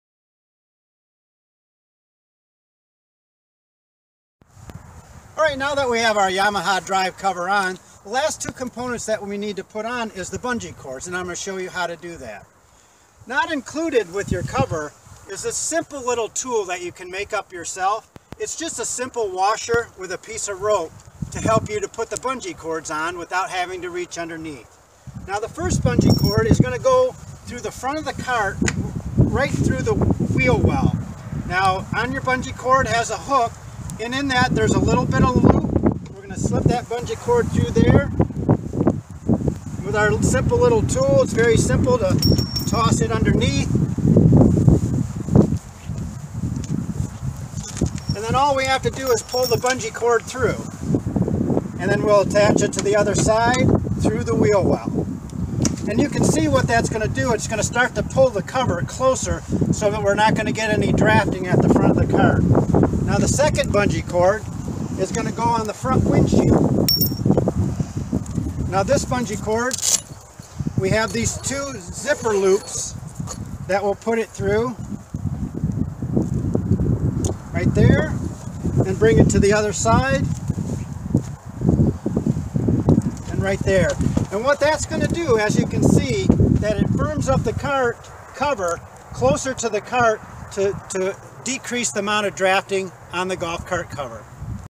Adding Bungee Cords to EZ Mag's Yamaha Drive Golf Cart Cover
Instructional video to show how to install two
"bungee cords to EZ Mag's Yamaha Drive  Golf Cart Cover (bungees are included, tool not included)